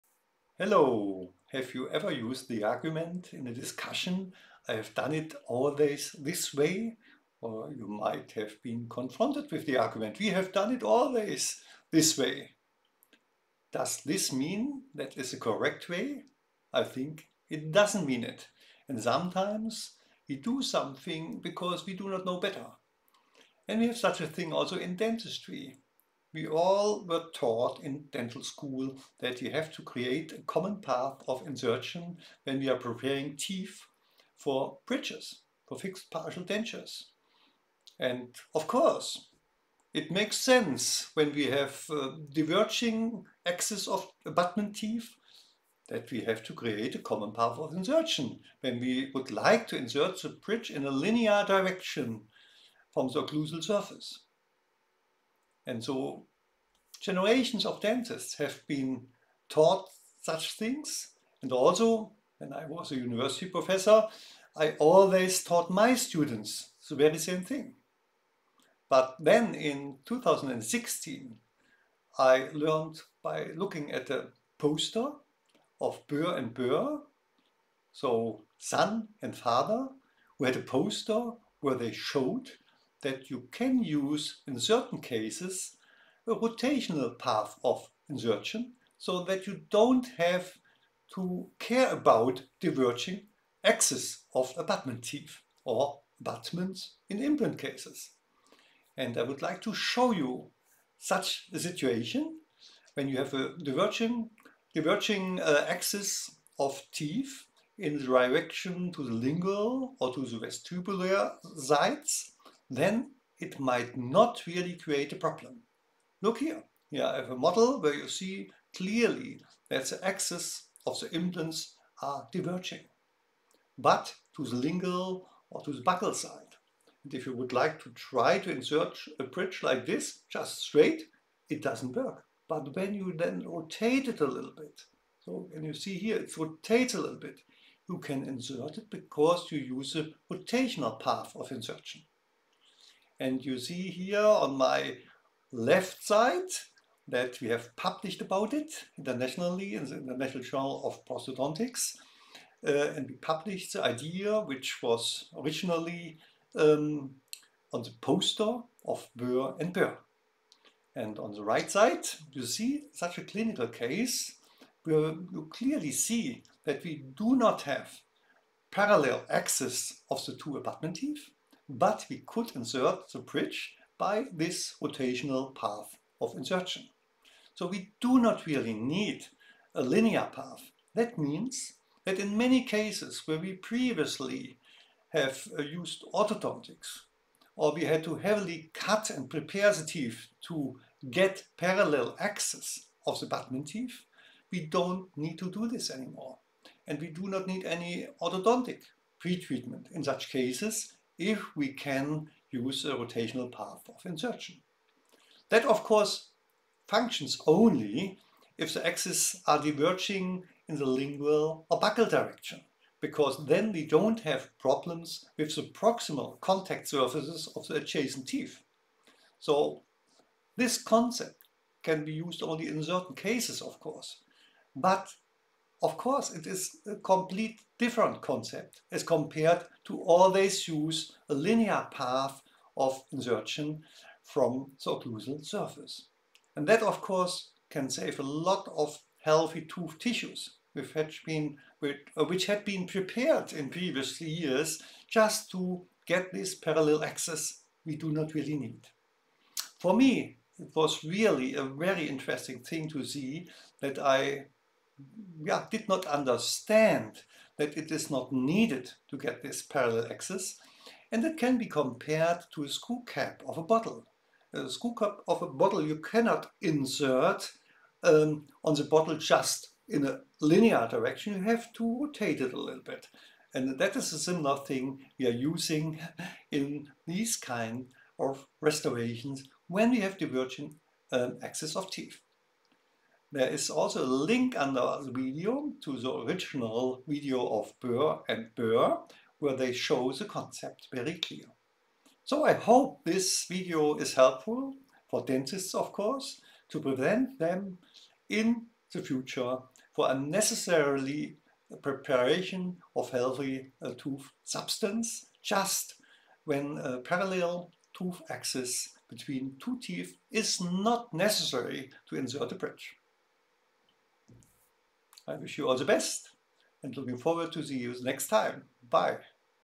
The common path of insertion - generations of dentists have been taught it a bit incorrectly
The common path of insertion when restoring teeth and implants with bridges is an untouched dogma, what generations of dentists have been taught - but what is wrong in certain cases. This video is intended to help dentists understand when tooth axes do not need to be parallelized and how this can preserve healthy tooth structure during abutment teeth preparations for dental bridges. The principle is also excellently illustrated in the video by W. Boer and M.W. Boer